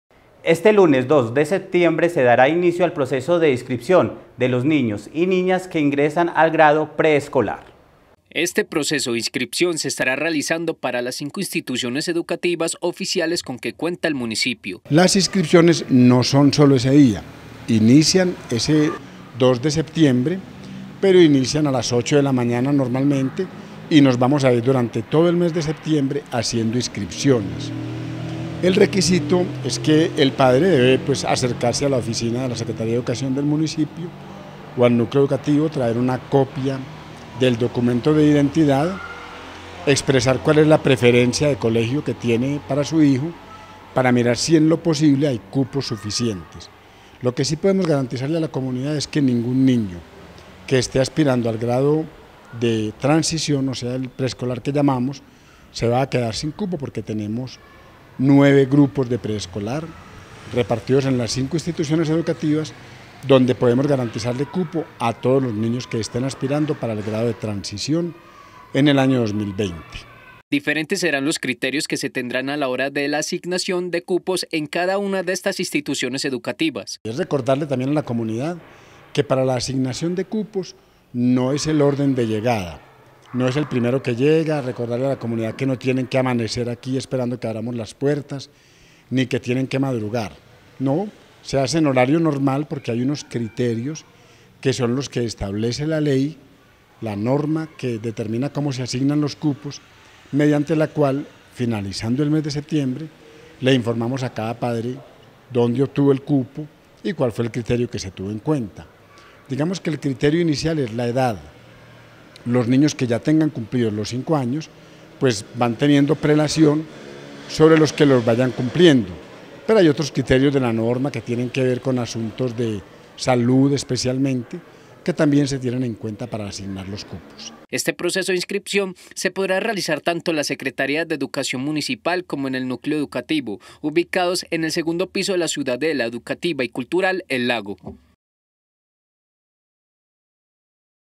PROCESO DE INSCRIPCIÓN PARA EL GRADO PREESCOLAR
El lunes 2 de septiembre se dará inicio al proceso de inscripción para los niños y niñas que ingresan al grado preescolar.